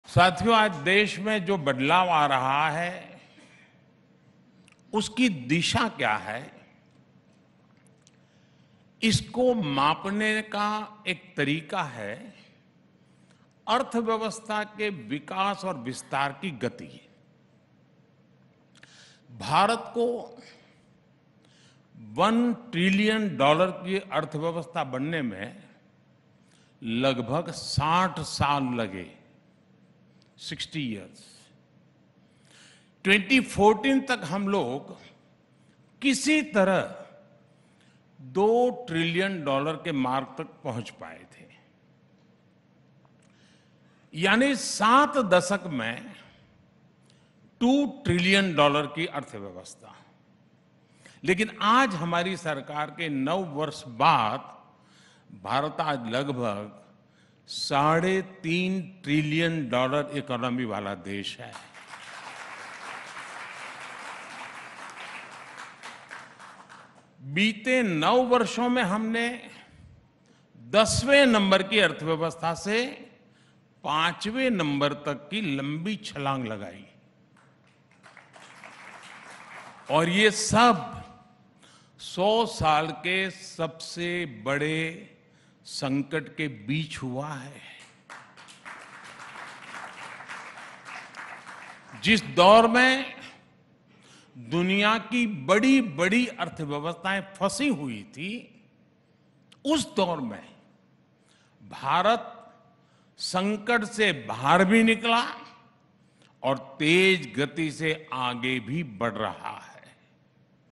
जानना चाहते हैं कि India किस तेज गति से आगे बढ़ रहा है तो इस आंकड़े पर नजर डालिए!
PM Modi addressed the Republic Summit. The country realized that India’s moment is now here, said PM Modi. Throwing light on this year’s theme ‘Time of Transformation’, PM Modi said that citizens can now witness the transformation on the ground which was envisioned 4 years earlier.